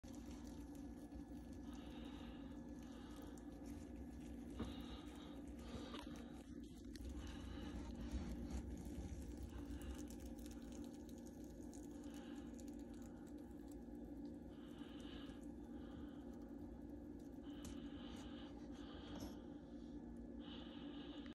Helping my corn snake shed
This is a YouTube shorts showing my corn snake Brownie. I went to my man cave and I caught my corn snake in the middle is shedding. His shed got stuck on his tail so I gave him a hand getting it off.